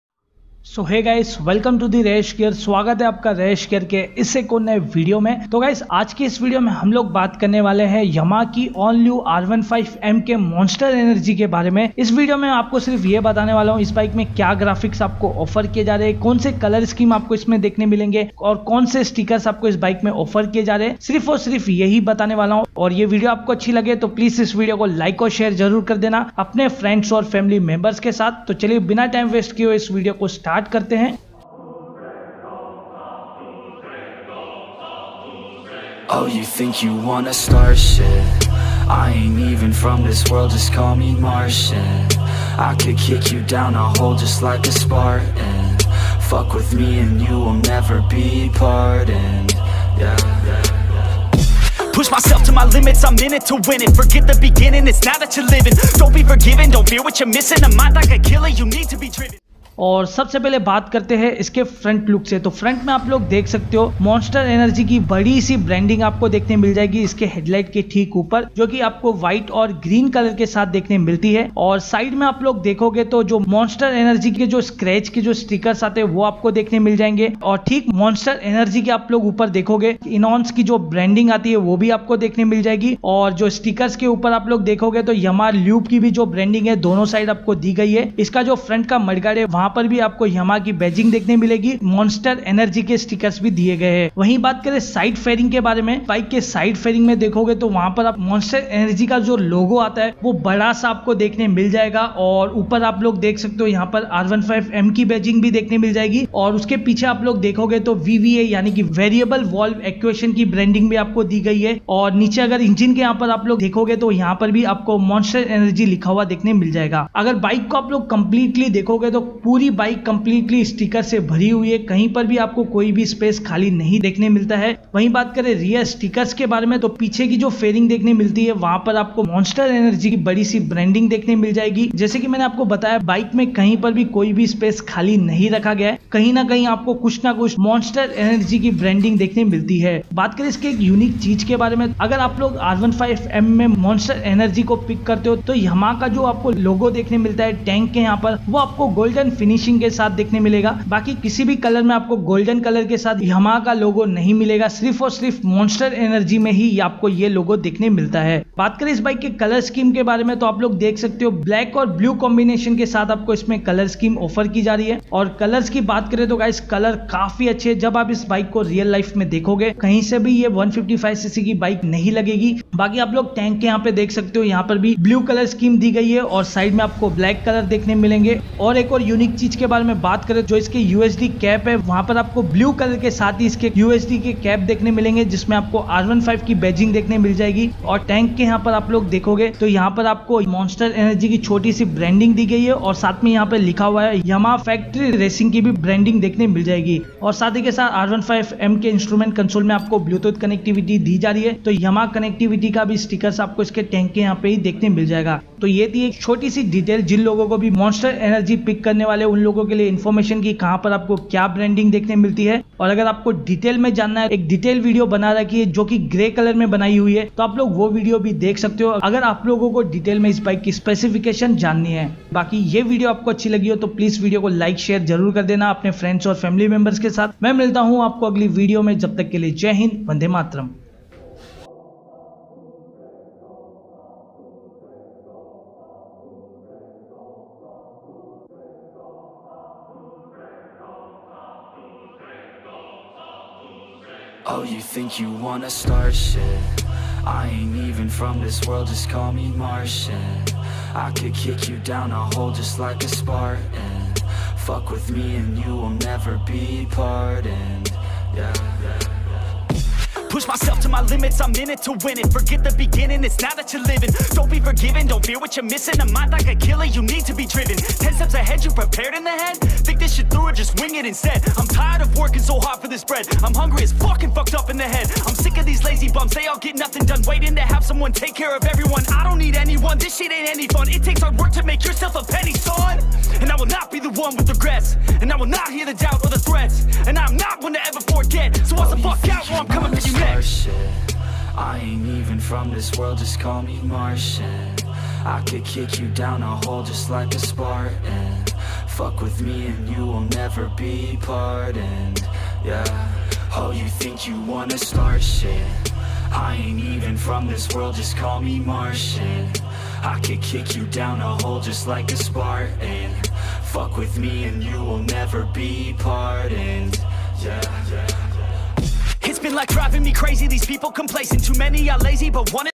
2021 Yamaha R15m Monster Energy Full Detailed Review | Better than r15 v4 ?
KNOW EVERYTHING ABOUT YAMAHA R15 M & R15 V4 IN DETAILING 👇🏻